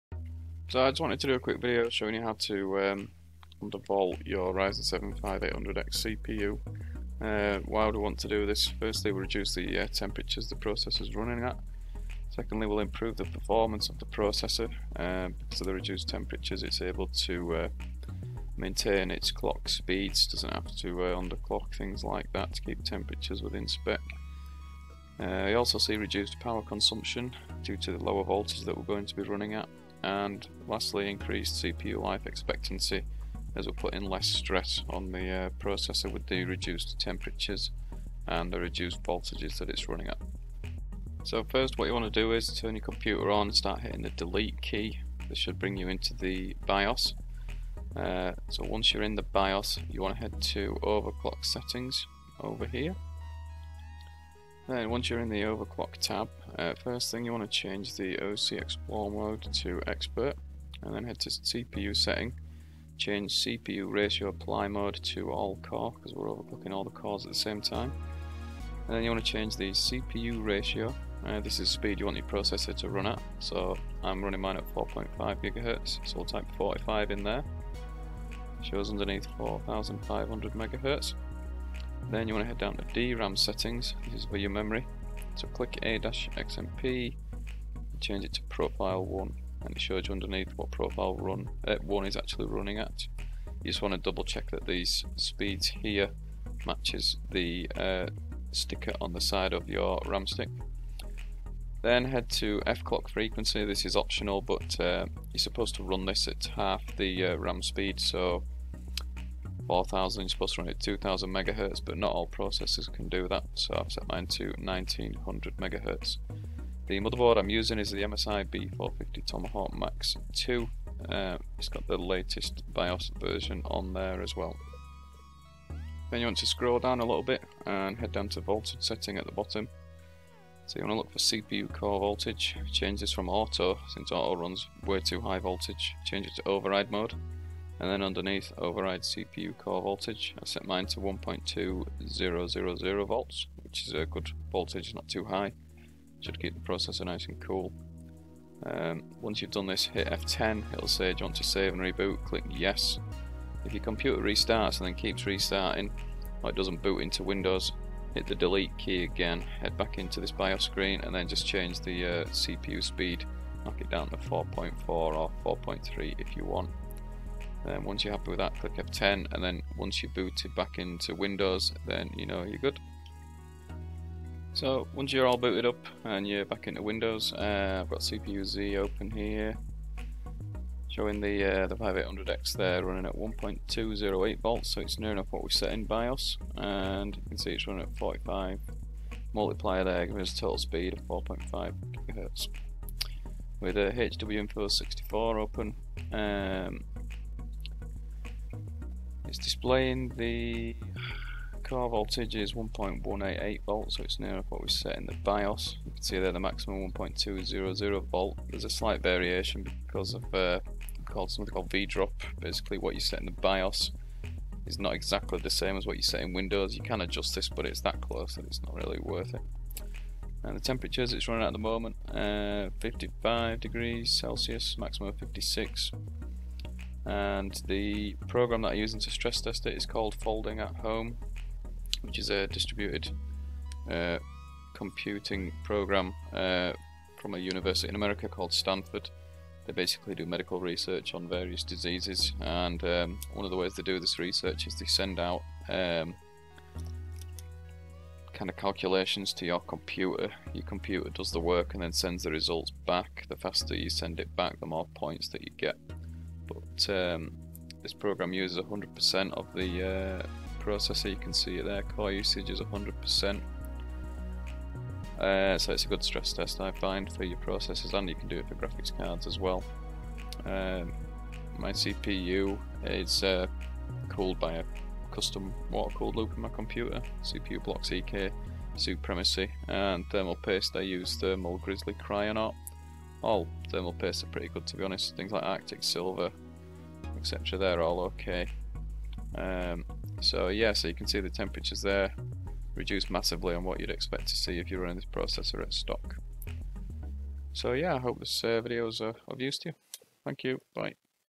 Ryzen 7 5800X Undervolt Guide - Lower Temps, Better Performance!
Timestamp (Chapters)

00:00 - Introduction/Undervolting Explained
00:41 - How To Apply An Undervolt In Bios
02:55 - Undervolt Applied In Windows Results
04:30 - Closing Thoughts

Hello

I wanted to do a quick video showing how to undervolt your Ryzen 7 5800X CPU.  Firstly why would we want to undervolt this CPU?  The Ryzen 7 5800X is a great processor, but due to so many cpu cores packed into a small space, the processor tends to run hot especially at stock.

In the video I give a brief intro as to why we undervolt the CPU, then go through the BIOS settings, then finally show the end result in windows.

By undervolting the CPU we gain several benefits in our gaming experience, including lower temps, reduced power consumption, better performance and longer cpu life.

My PC Specifications:

CPU - Ryzen 7 5800X 8c/16t
GPU - Nvidia RTX 3080 FE
Motherboard - MSI B450 Tomahawk Max II
RAM - DDR4 2x 16GB 4000mhz
PSU - Cosair TX750M
OS - Windows 10 64bit
Cooling - Custom watercool loop

This is how I run my 5800X and have been running it perfectly fine for months.  I have produced this video as a guide, you can alter the settings if you wanted to.

I take no responsibility for the settings in this vieo on your pc.  You follow this guide on your own responsiblity.  It is up to the individual user to ensure you use good quality parts, and you are aware as to the risk of altering your settings within the PC.

If you do not understand CPU overclocking, I advise you to read up on the process before watching this video.  I offer this video as a guide, to share my overclocking experience.

You should always stress test any CPU overclock to ensure stability, I use F@H to stress test my CPU and GPU.